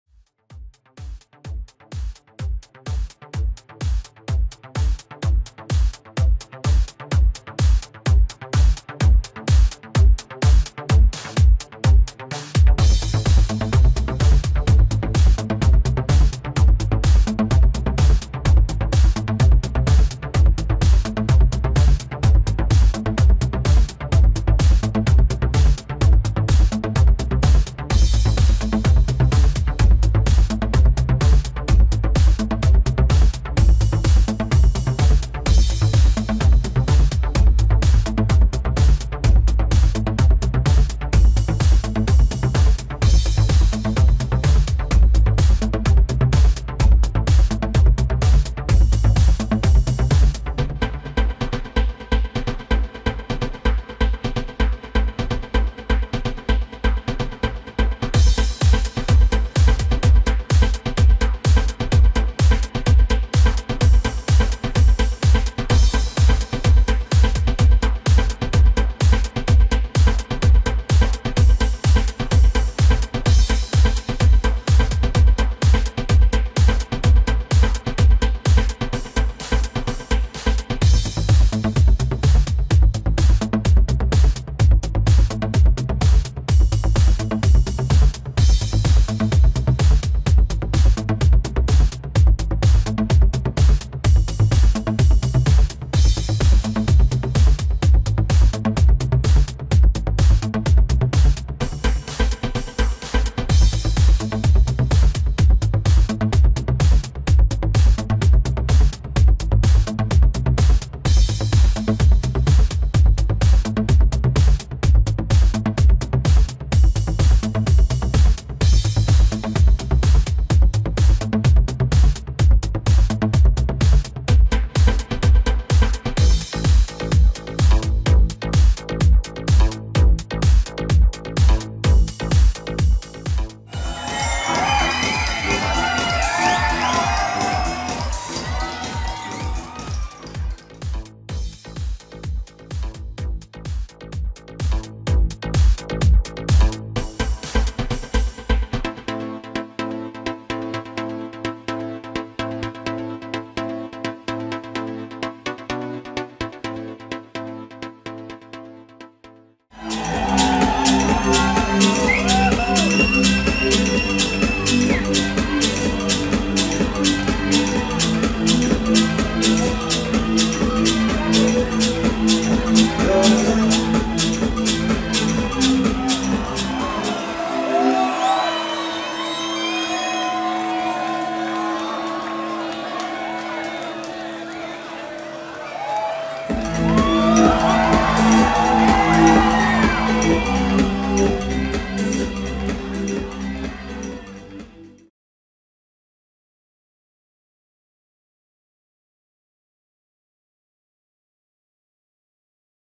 Airport 14.-17.12.2011 Marek Hemmann
Die Airport Woche vom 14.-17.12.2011 - Marek Hemmann war der absolute Burner!

Movie by webflasher - Soundtrack wurde vom Urheber freigegeben.